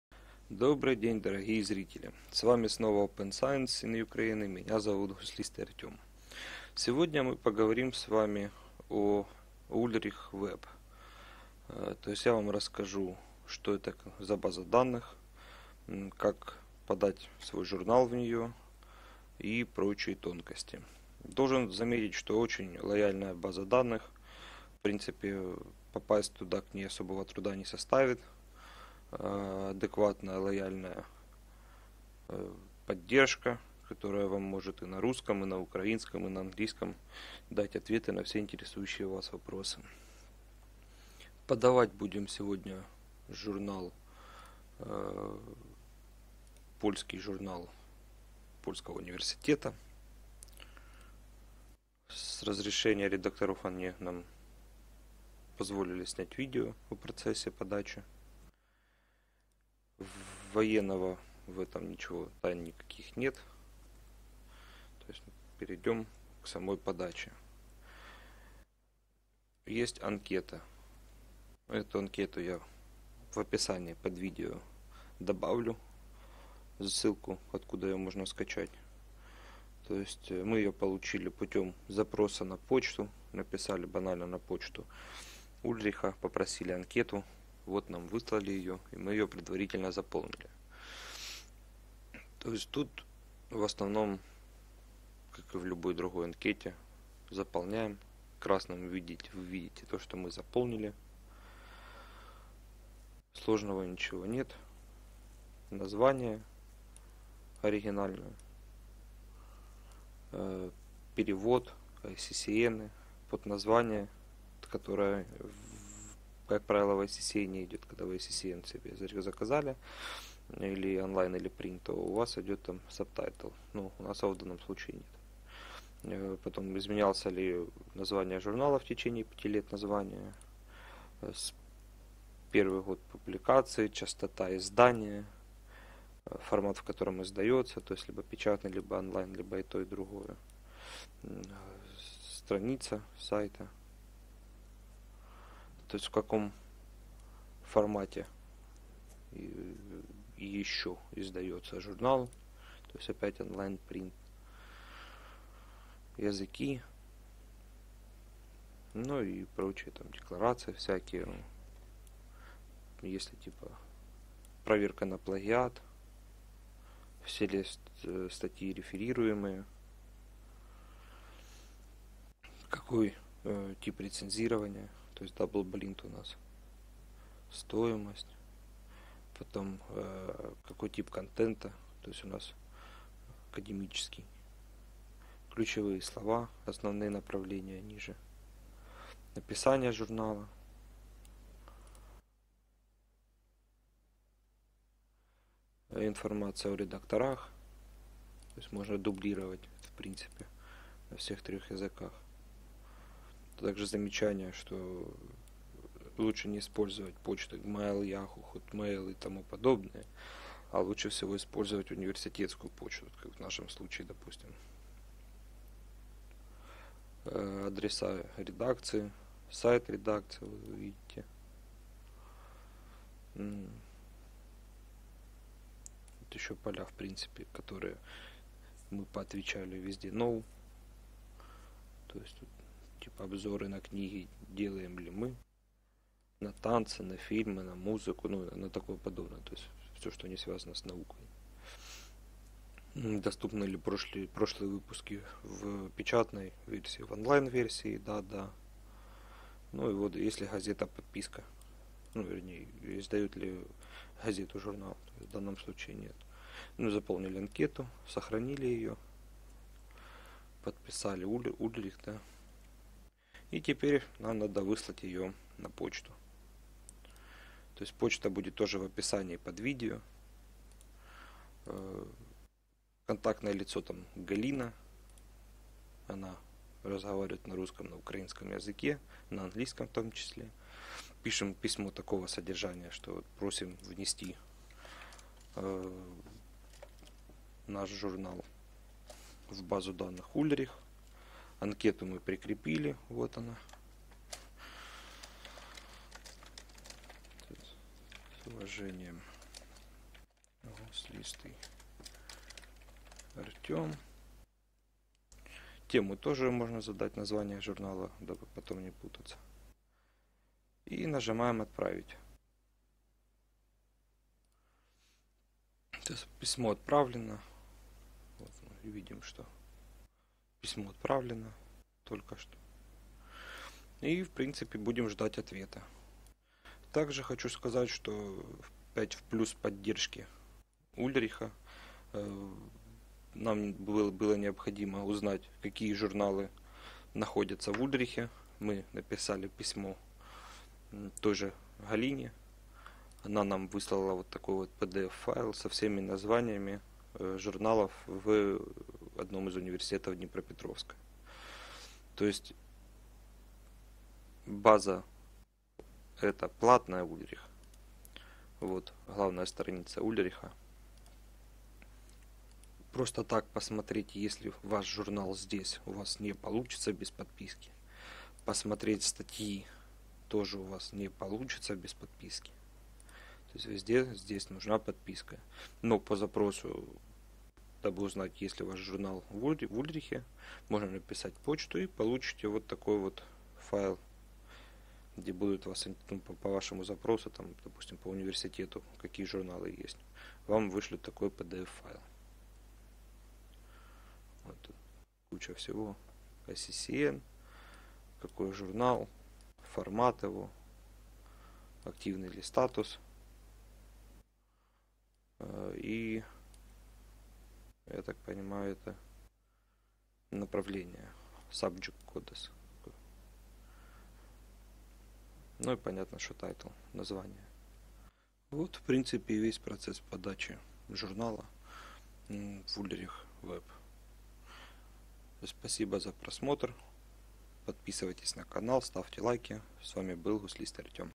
Регистрация журнала в Ulrich'sweb (ProQuest)
В этом видео мы покажем Вам как зарегистрировать свой научный журнал в международную базу данных Ulrich'sweb (ProQuest)